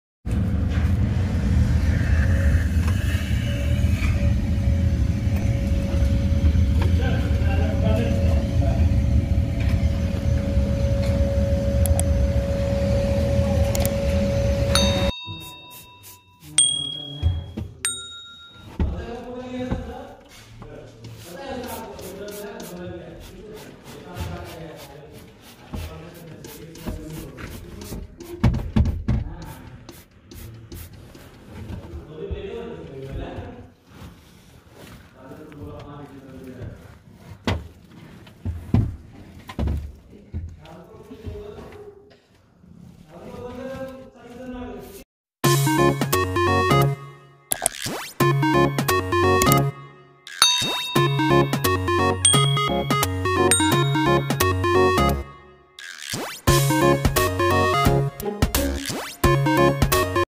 How dirty can an airduct be if not sealed while building construction is going on?
pigion droppings or shit or poop and even dead pigion can sit on airduct if taken for granted...